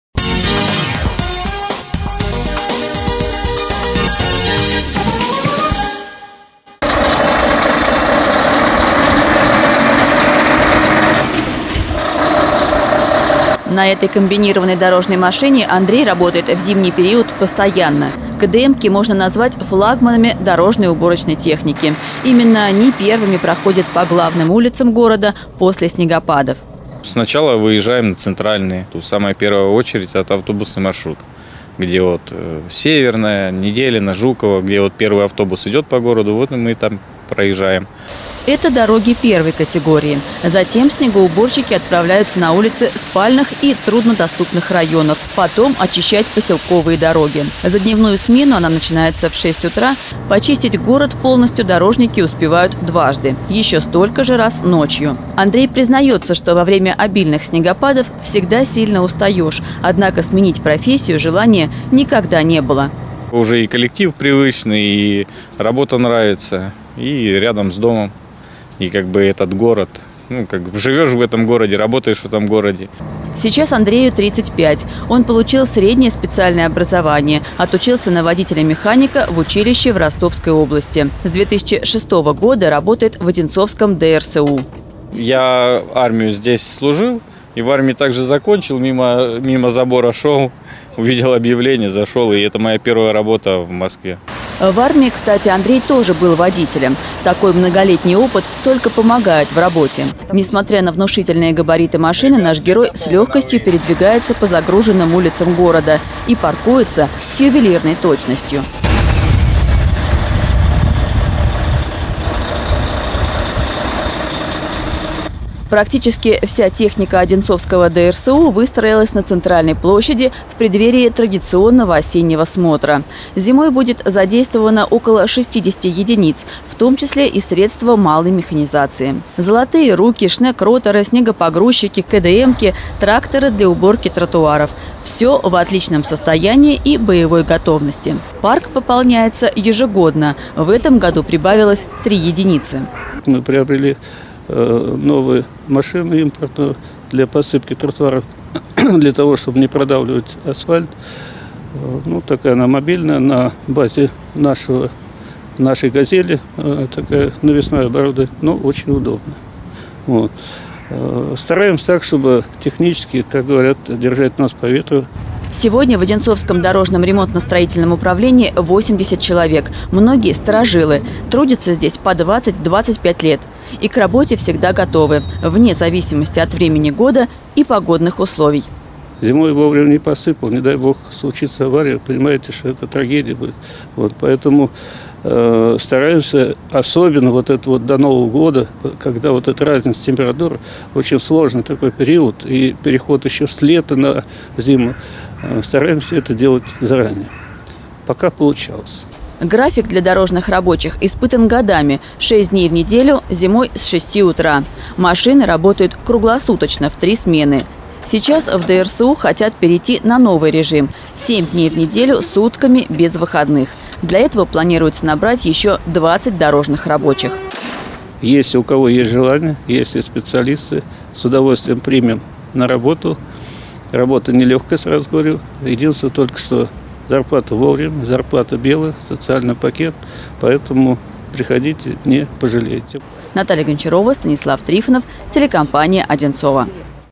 День работников дорожного хозяйства
День работников дорожного хозяйства с 2000 года отмечается в третье воскресенье октября. В этом году он выпал на 17-е число. В преддверии профессионального праздника съемочная группа телеканала побывала в Одинцовском ДРСУ и узнала о работе его сотрудников.